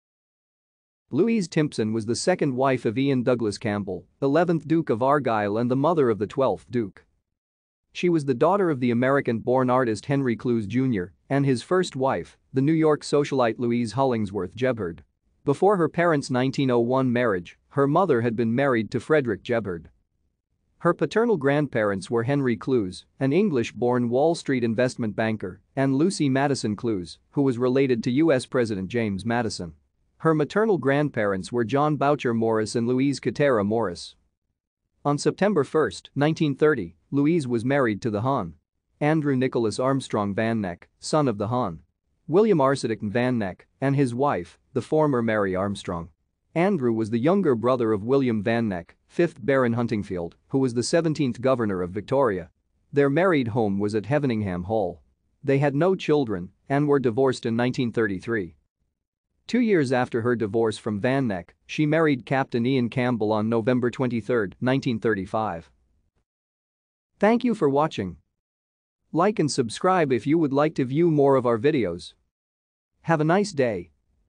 Louise Timpson
Louise Timpson  was the second wife of Ian Douglas Campbell, 11th Duke of Argyll and the mother of the 12th Duke.